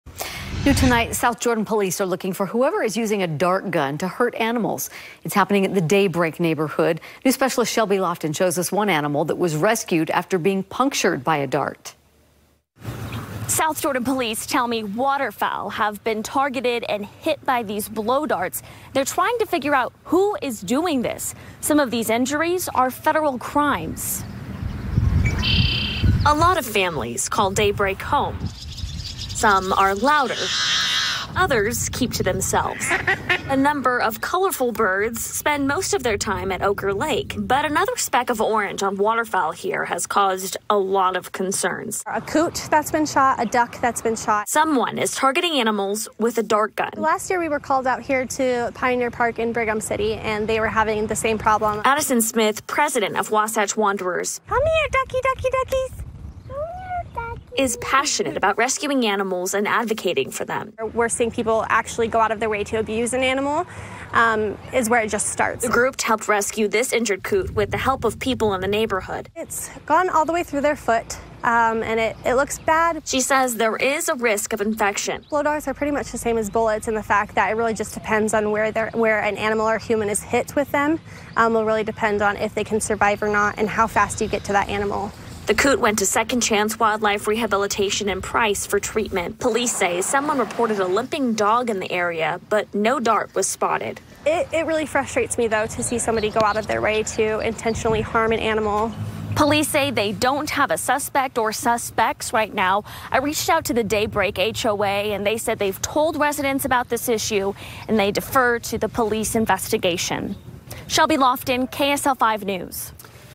South Jordan police search for person targeting, hitting animals with blow darts
Several colorful birds make their home at the Oquirrh Lake in the Daybreak neighborhood, but someone is targeting them with a dart gun, police say.